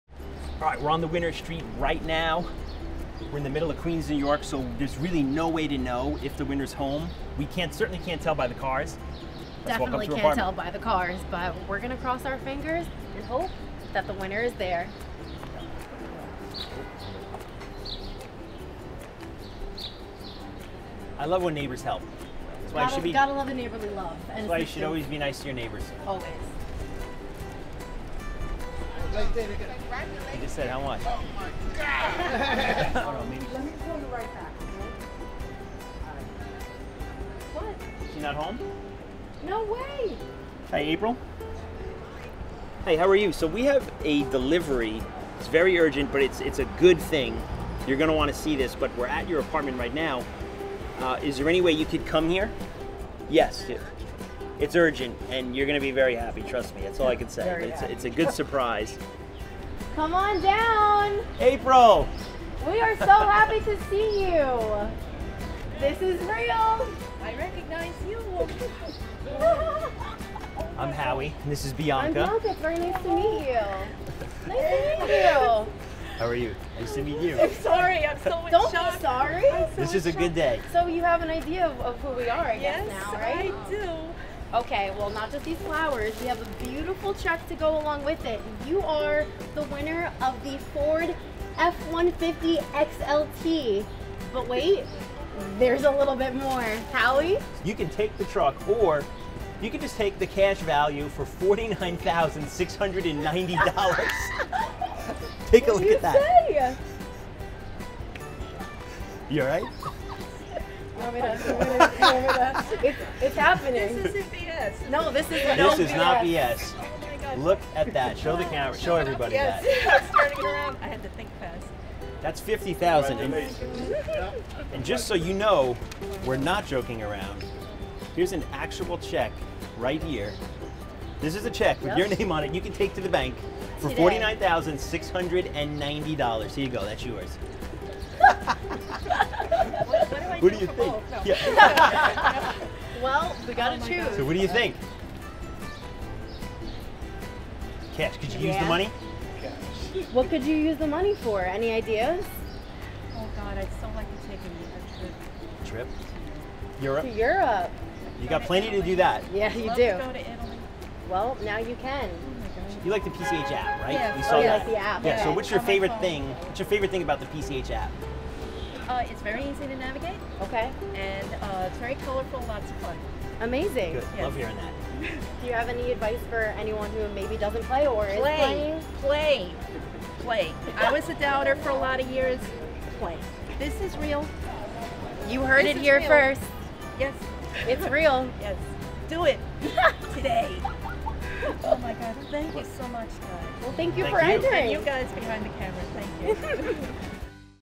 PCH Sweepstakes Winner: April H. from Ridgewood, NY Wins the Ford F-150 XLT valued at $49,690.00!
Watch the moment we surprised New Yorker, April with a PCH prize! We come to doors across America - yours could be next